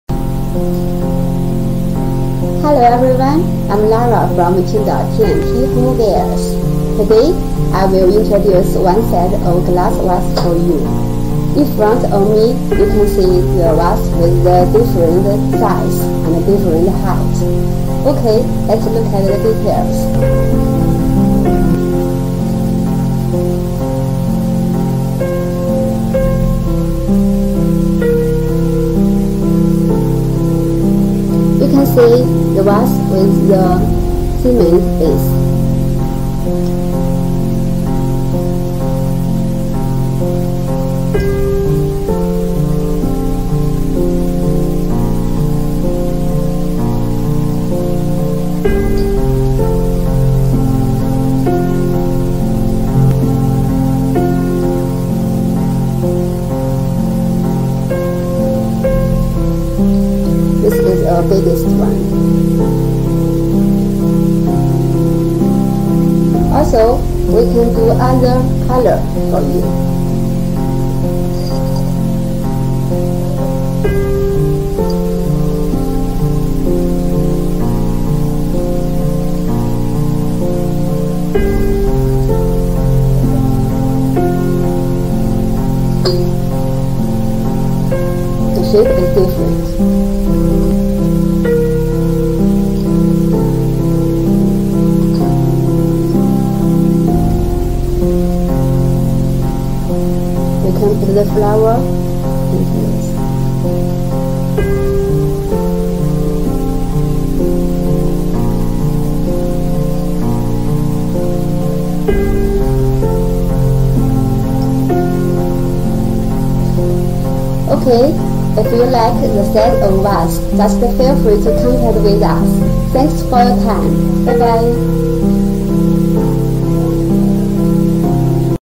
Creative Design
Life is very heavy，so we must light our life. Life is a kind of life attitude. The creativity,  Cement together with glass, looks greatly humble, then becomes fashionable tonal style implements. The circular mouth is round and smooth. Separate two parts can be easily picked up a change water. In the meanwhile, the bottom is not easily to shake.